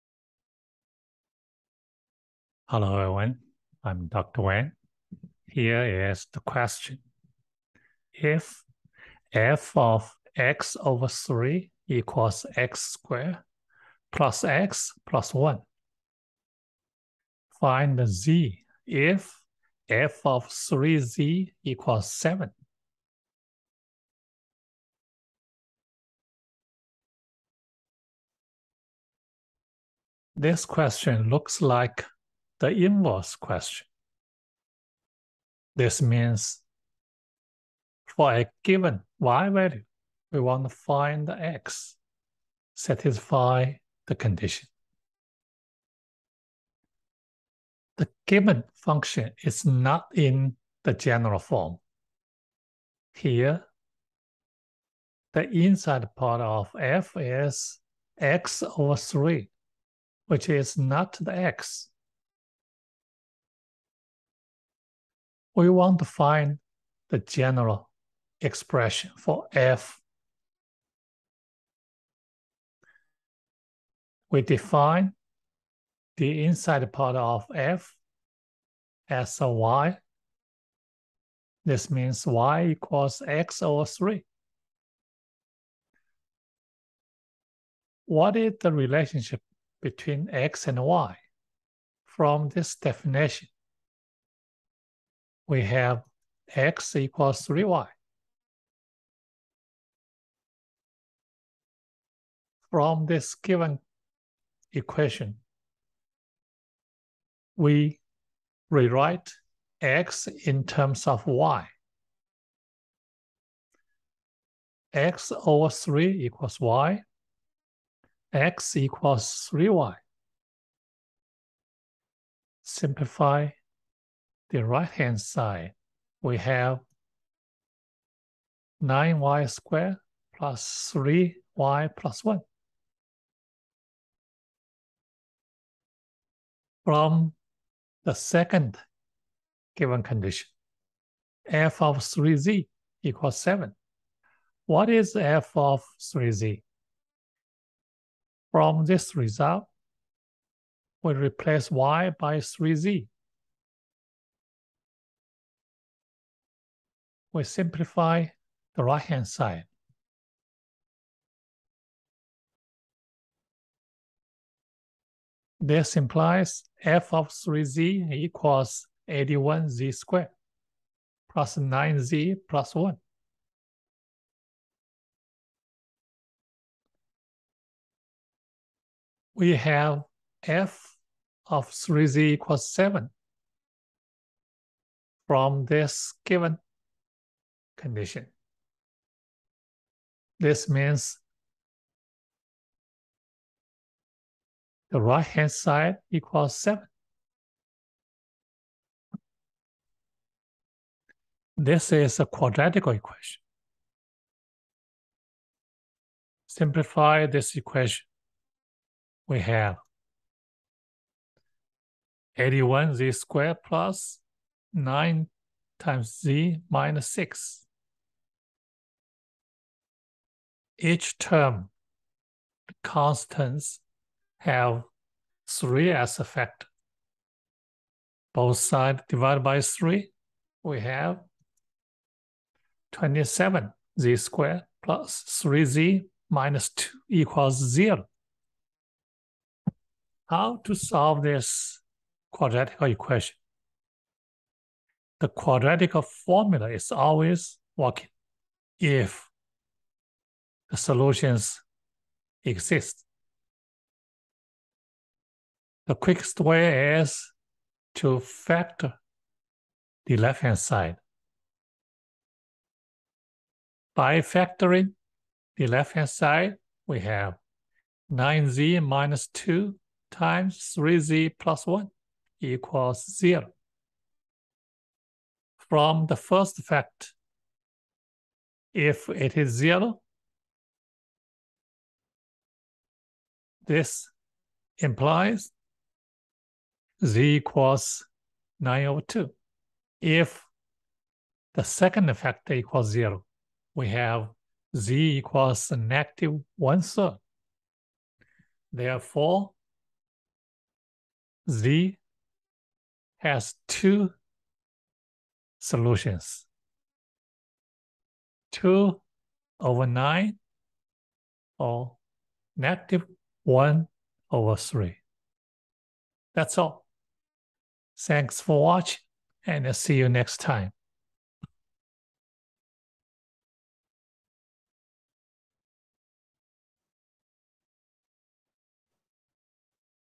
A Functional Problem, AMC Question
This is a functional problem. Given a function value, what is z value? In this video, I'll show you how to find a general expression for this function and then solve a quadratic equation to find the z value.